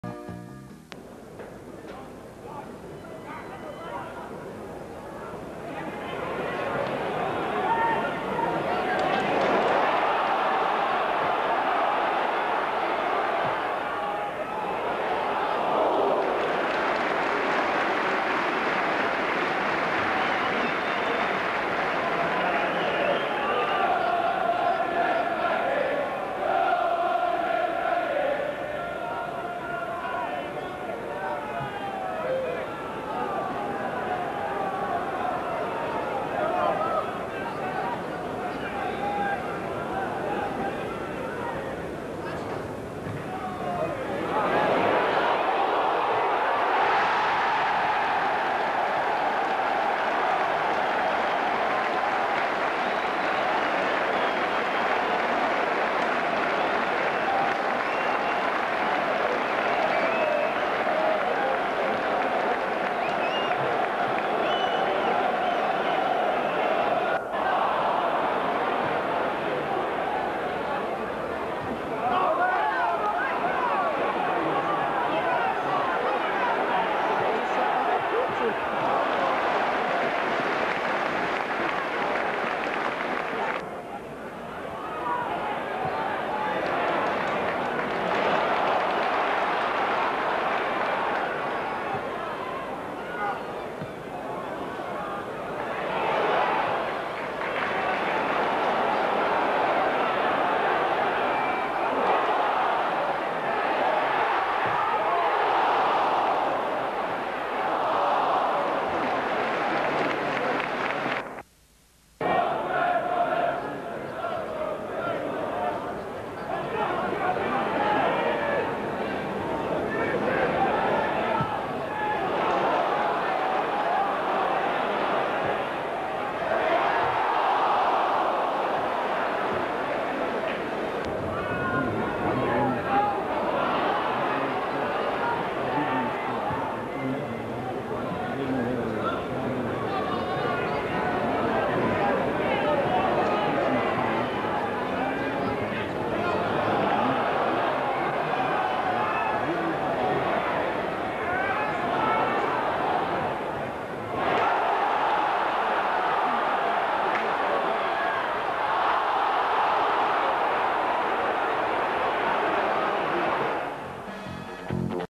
1990-91 West Bromwich Albion v Charlton
Boxing Day fare at The Hawthorns 1990. Woking were just around the corner.